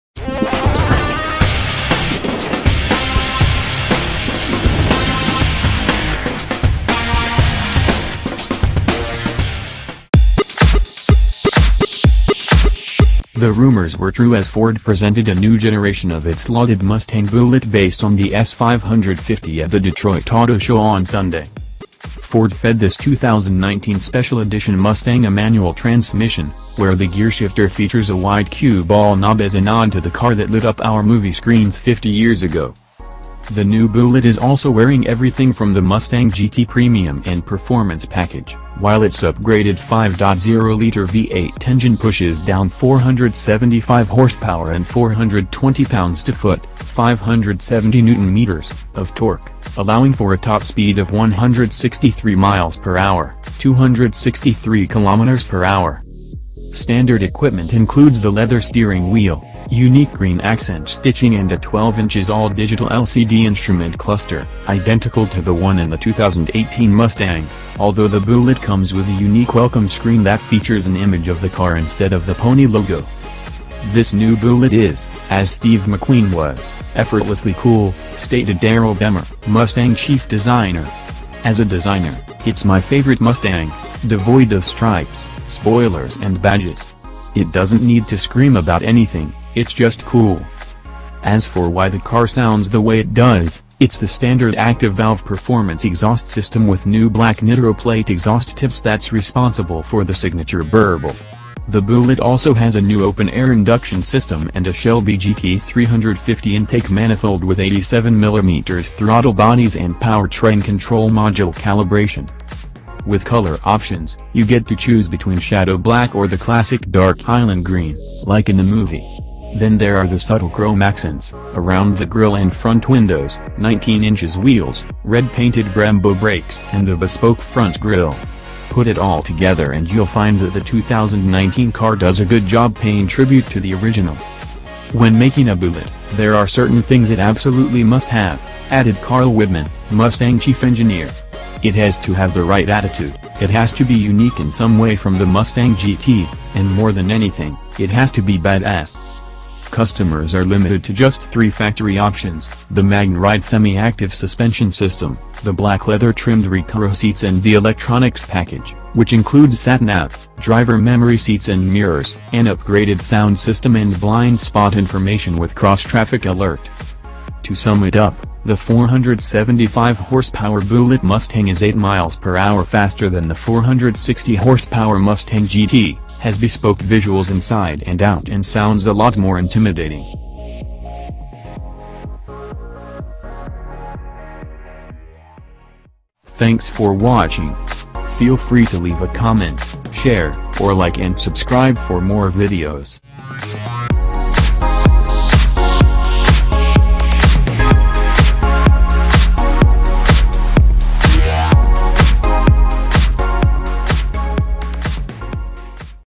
Must Watch!!! 2019 Ford Mustang Bullitt Unveiled Interior Exterior | Loud Engine
Welcome to Loud Engine Channel (Car News and Review)
This time we will share to you about 2019 Ford Mustang Bullitt Unveiled Interior Exterior.

The rumors were true as Ford presented a new generation of its lauded Mustang Bullitt based on the S-550 at the Detroit auto show on Sunday.

Ford fed this 2019 special-edition Mustang a manual transmission, where the gear shifter features a white cue ball knob as a nod to the car that lit up our movie screens 50 years ago.

The new Bullitt is also wearing everything from the Mustang GT Premium and Performance Package, while its upgraded 5.0-liter V8 engine pushes down 475 HP and 420 lb-ft (570 Nm) of torque, allowing for a top speed of 163 mph (263 km/h).

Standard equipment includes the leather steering wheel, unique green accent stitching and a 12" all-digital LCD instrument cluster (identical to the one in the 2018 Mustang), although the Bullitt comes with a unique welcome screen that features an image of the car instead of the pony logo.

"This new Bullitt is, as Steve McQueen was, effortlessly cool," stated Darrell Behmer, Mustang chief designer. "As a designer, it's my favorite Mustang - devoid of stripes, spoilers and badges. It doesn't need to scream about anything - it's just cool."

As for why the car sounds the way it does, it's the standard active valve performance exhaust system with new Black NitroPlate exhaust tips that's responsible for the signature burble. The Bullitt also has a new Open Air Induction System and a Shelby GT350 intake manifold with 87 mm throttle bodies and powertrain control module calibration.

With color options, you get to choose between Shadow Black or the classic Dark Highland Green (like in the movie). Then there are the subtle chrome accents (around the grille and front windows), 19" wheels, red-painted Brembo brakes and the bespoke front grille. Put it all together and you'll find that the 2019 car does a good job paying tribute to the original.

"When making a Bullitt, there are certain things it absolutely must have," added Carl Widmann, Mustang chief engineer. "It has to have the right attitude, it has to be unique in some way from the Mustang GT, and more than anything, it has to be bad ass."

Customers are limited to just three factory options, the MagneRide semi-active suspension system, the black leather-trimmed Recaro seats and the Electronics Package, which includes sat-nav, driver memory seats and mirrors, an upgraded sound system and Blind Spot Information with Cross-Traffic Alert.

To sum it up, the 475 HP Bullitt Mustang is 8 mph faster than the 460 HP Mustang GT, has bespoke visuals inside and out and sounds a lot more intimidating.

2019 Ford Mustang Bullitt Unveiled Interior Exterior